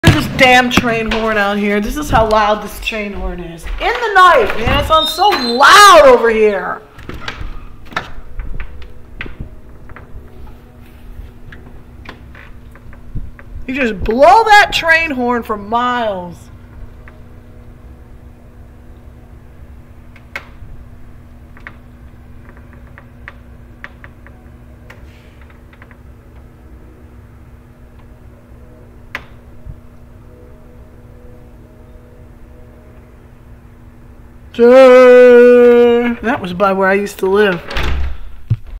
LOUD train horn after midnite
Trains have come through this area for decades, blasting that horn for miles--- sometimes at 3:30 a.m. and later.